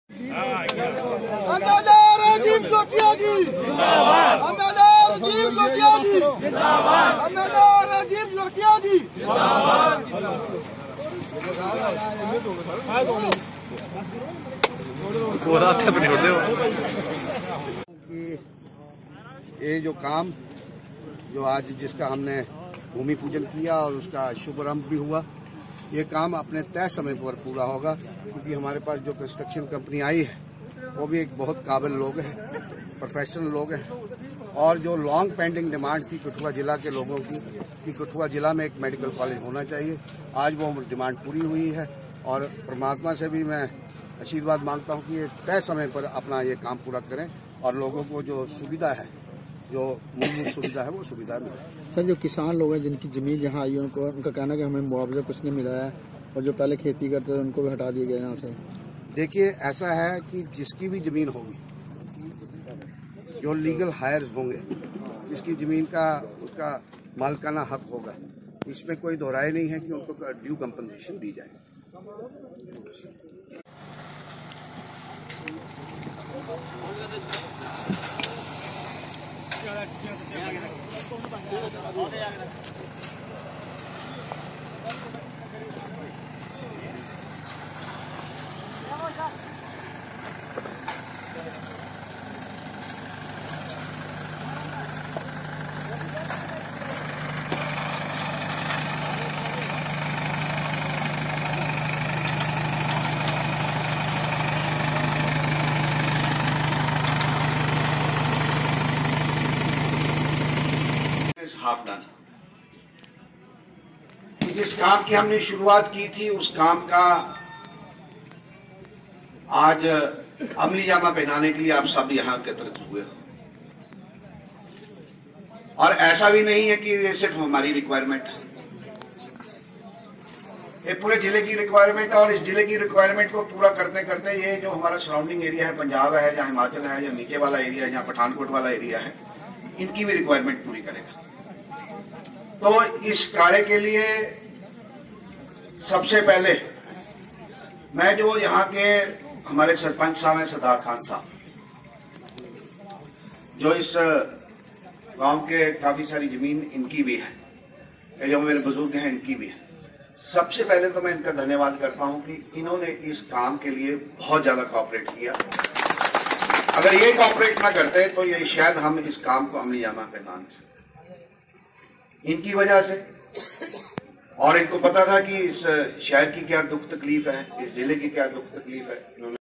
Rajiv Jasrotia MLA kathua
आज विधायक राजीव जसरोटिया जी ने  मेडिकल कॉलेज कठुआ का भूमिपूजन किया